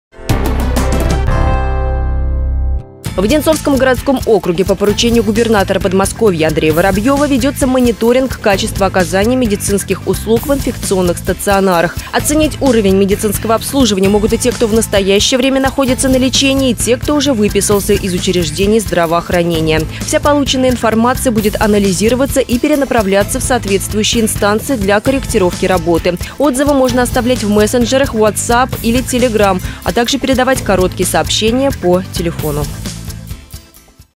Ваше мнение важно!
В Одинцовском городском округе по поручению Губернатора Подмосковья Андрея Воробьева ведётся мониторинг качества оказания медицинских услуг в инфекционных стационарах.